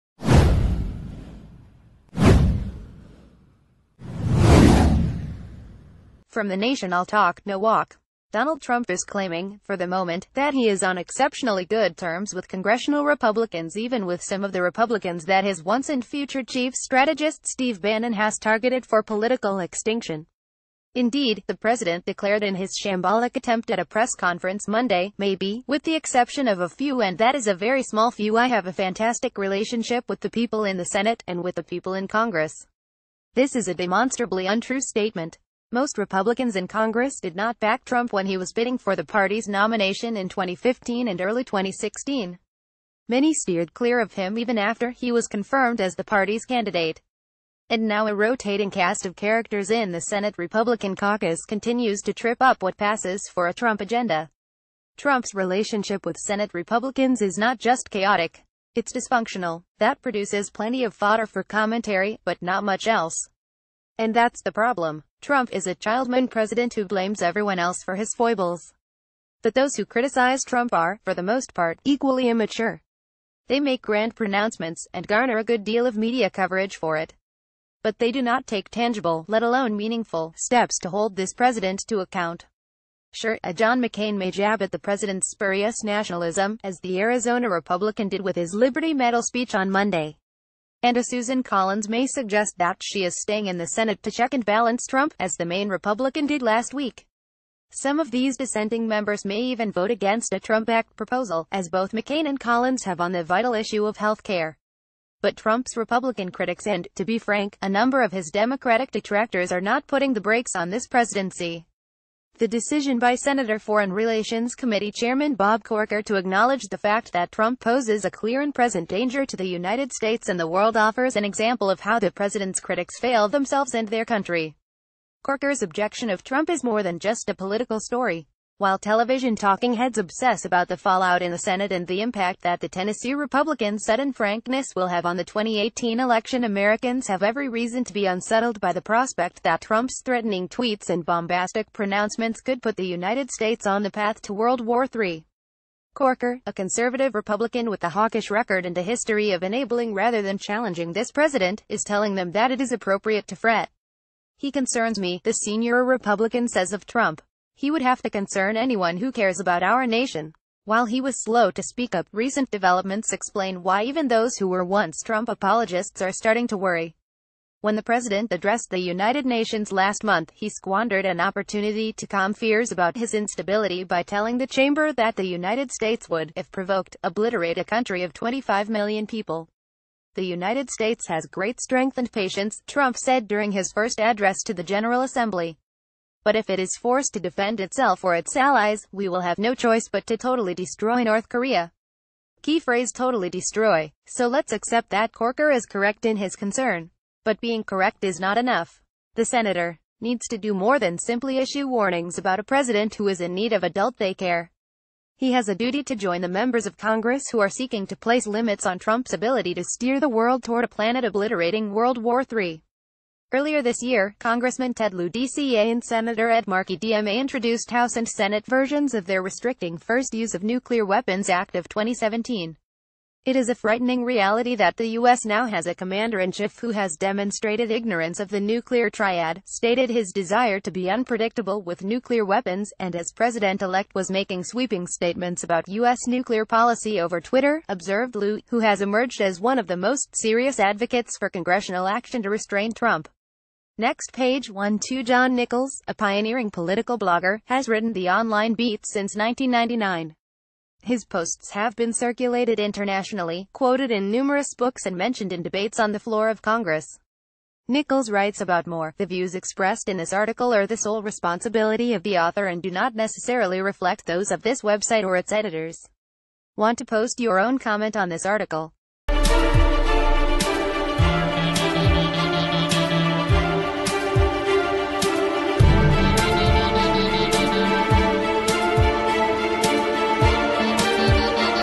Breaking News | Article: actually, no, bob corker and john mccain are doing nothing to hold trump a
Article: actually, no, bob corker and john mccain are doing nothing to hold trump accountable

From The Nation All talk, no walk. Donald Trump is claiming, for the moment, that he is on exception...